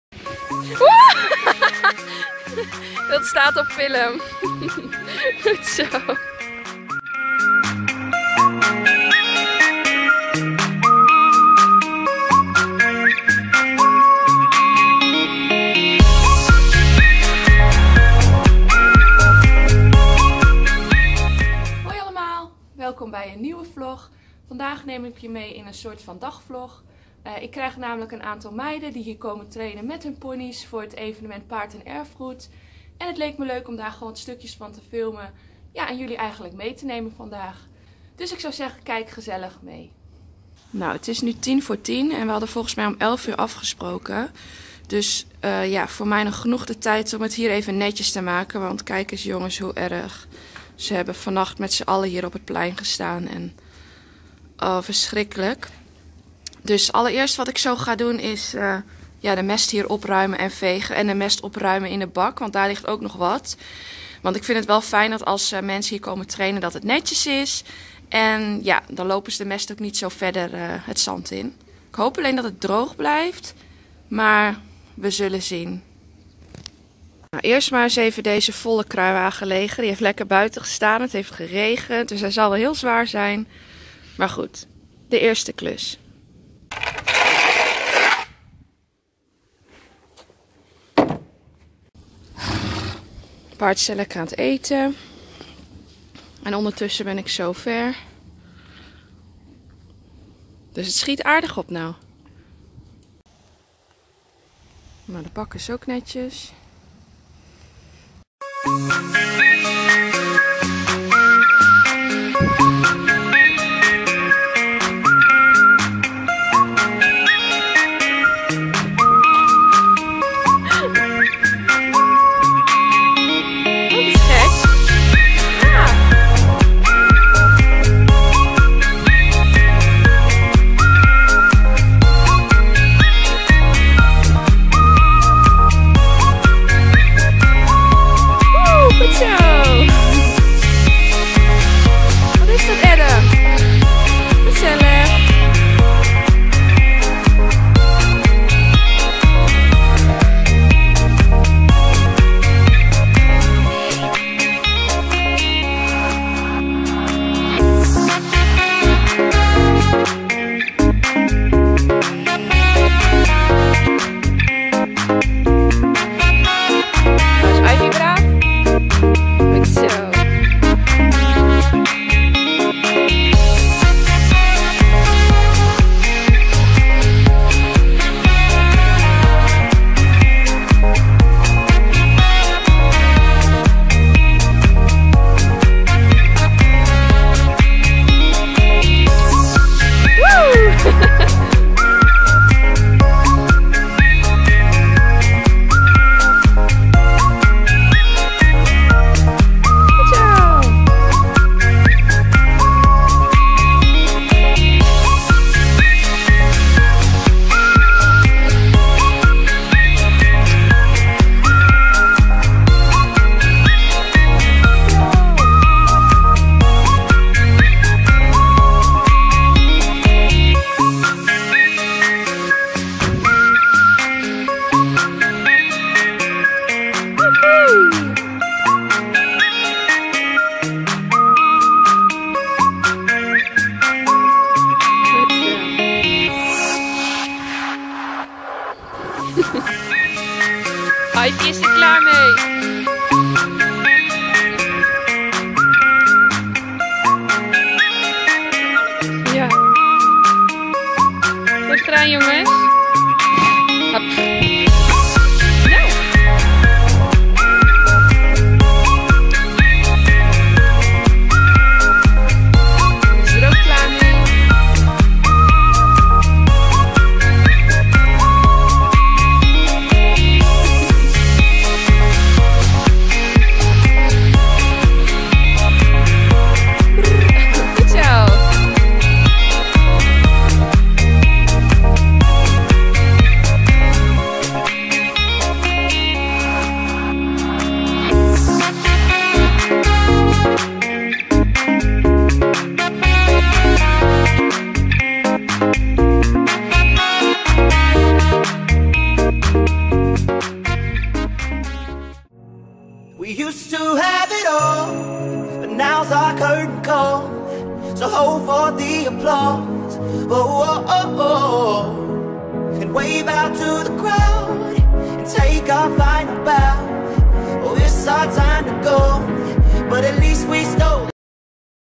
Oefenen voor Paard & Erfgoed! | Joyce & Jaydee de Vos
Op zondag 3 september geef ik een clinic/demonstratie op het evenement Paard & Erfgoed, bij Hoeve Boschoord in Doldersum (Drenthe). Dit jaar neem ik even geen eigen paarden mee, omdat ik het heel erg druk heb (heb daarvoor ook 2 dagen show) en Jaydee is op vakantie, dus even keuzes maken af en toe!
Ik ga dan tijdens dit evenement 5 combinaties lesgeven tijdens de clinic/demonstratie. Dit zijn kinderen van de organisatie en cliënten van Hoeve Boschoord/Stichting Trajectum.
We deden even een gezamelijke training vooraf, omdat het voor hun best wel spannend is,  zo kon ik ook even kijken hoe de paarden reageerden zodat ze veilig mee konden doen tijdens de demonstratie ;) Uiteraard hebben we niet alle obstakels geoefend, zodat er ook nog een beetje een verrassing is op het evenement zelf en we goed kunnen demonstreren wat je precies moet doen als je paard het spannend vindt of iets nog niet durft.
Veel kijkplezier en wie weet tot op Paard & Erfgoed op zondag 3 september!